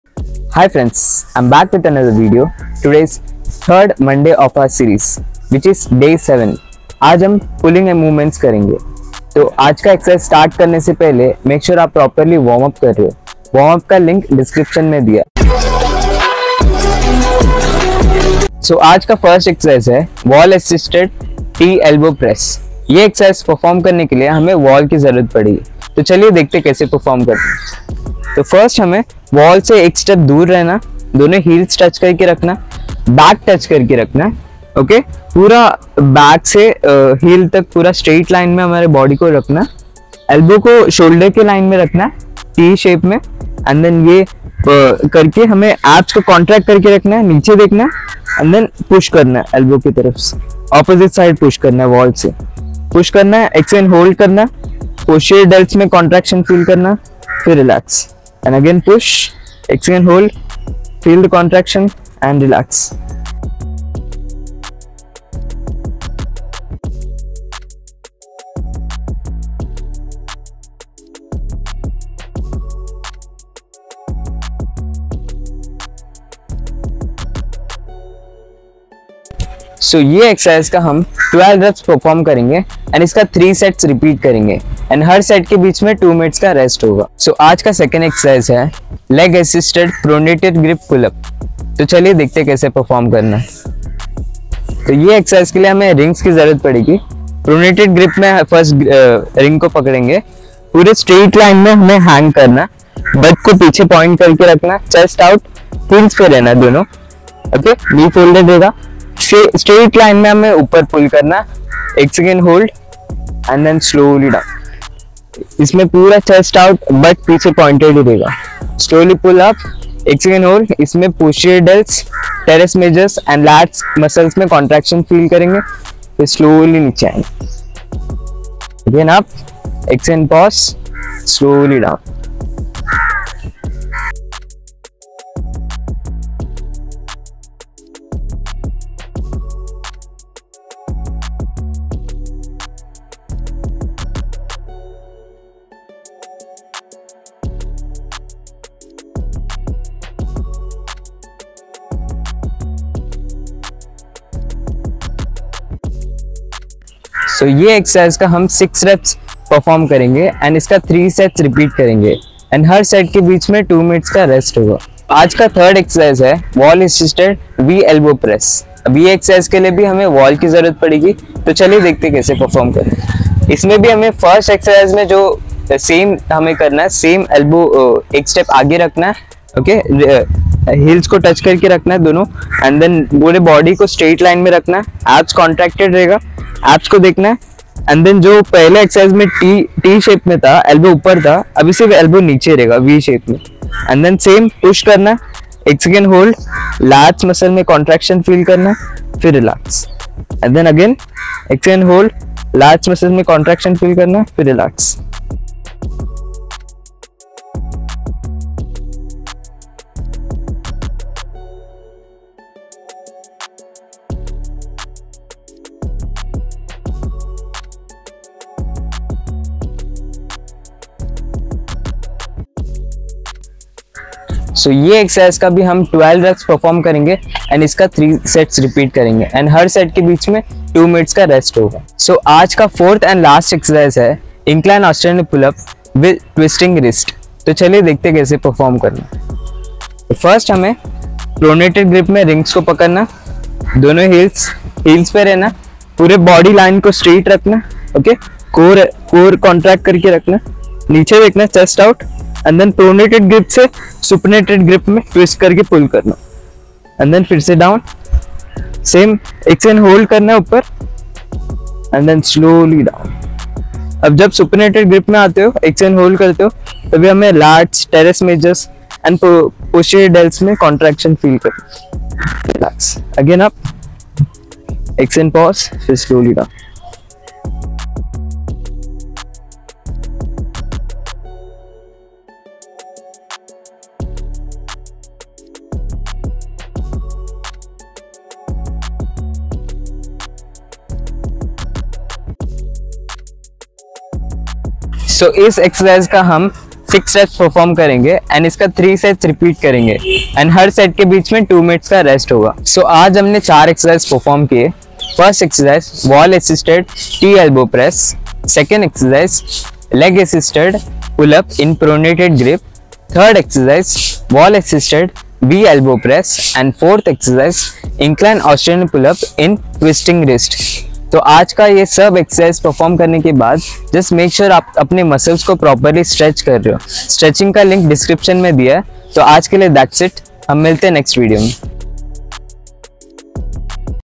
CALISTHENICS DAY 7 (PULLING MOVEMENTS)
PULLING MOVEMENTS

HERE IS OUR PULLING EXERCISE
1.WALL ASSISTED T EBLOW PRESS
2. LEG ASSISTED PULL UPS (PRONATED GRIP)
3. WALL ASSISTED V EBLOW PRESS
4. INCLINE AUSTRALIAN PULL UPS WITH TWISTING WRIST